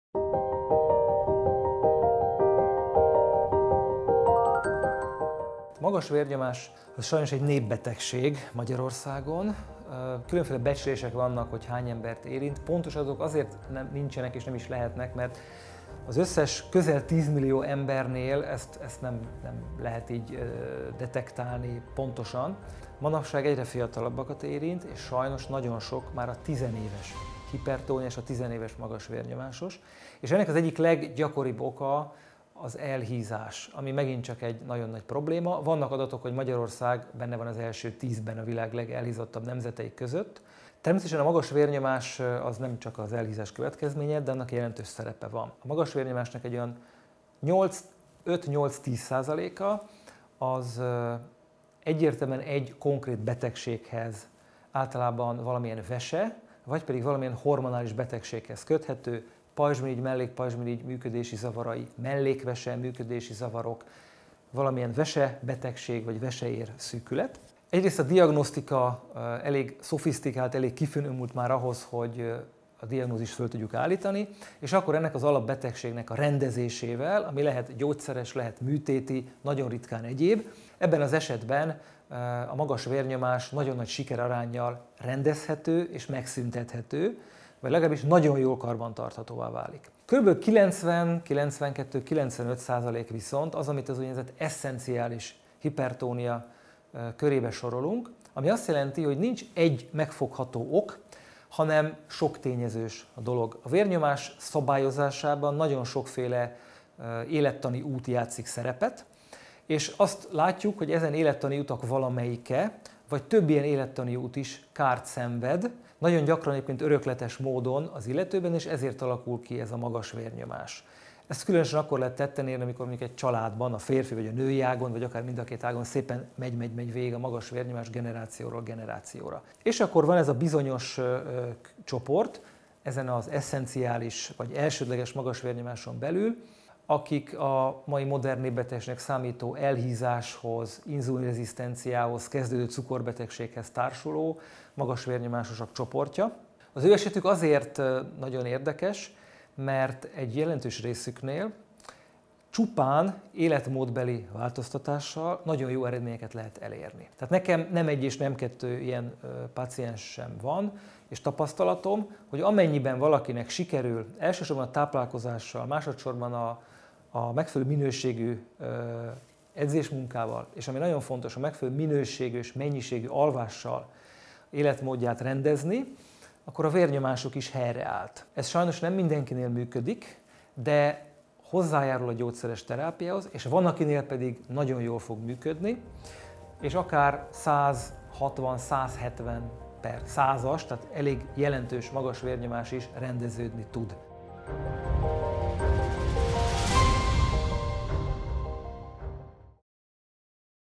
A magas vérnyomásról – dr. Pap Csaba Zsolt kardiológus-belgyógyász
A magas vérnyomás Magyarországon népbetegség, amely a fiatalokat is egyre nagyobb számban érinti. Oka lehet például a pajzsmirigy vagy a mellékvese működési zavara, vesebetegség, veseér-szűkület. Ezekben az esetekben a magas vérnyomás jól kezelhető. Abban az esetben, ha nincs egyértelmű oka a magas vérnyomásnak, például öröklődik vagy elhízásból eredeztethető, esszenciális hipertóniáról beszélünk.

Videónkban dr. Pap Csaba Zsolt kardiológus beszél a magas vérnyomásról.